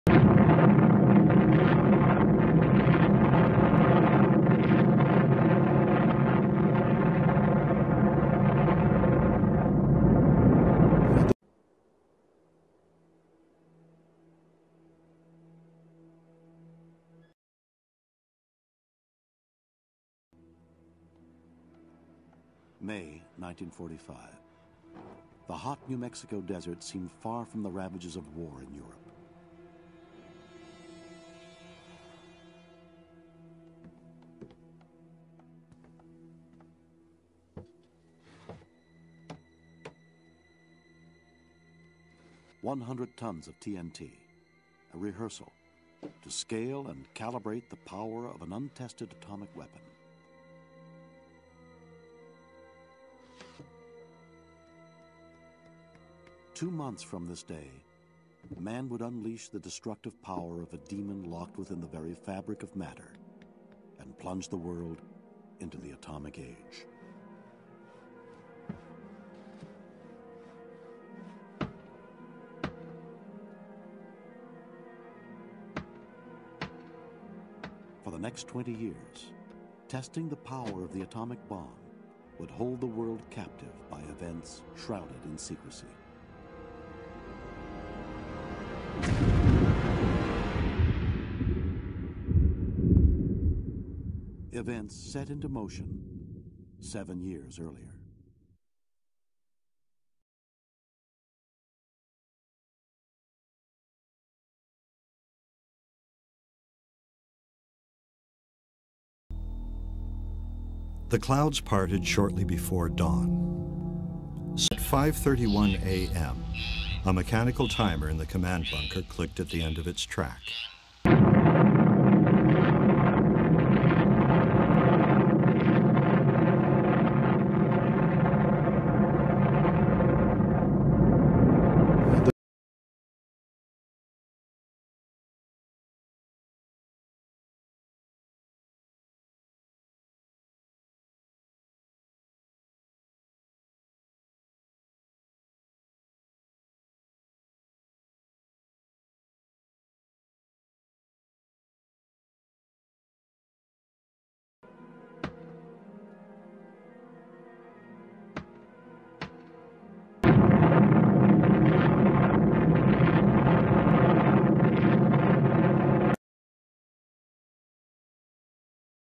Trinity the first 20 Kiloton bomb Compared to a 100 ton TNT Blast
atomic bomb compared to TNT explosion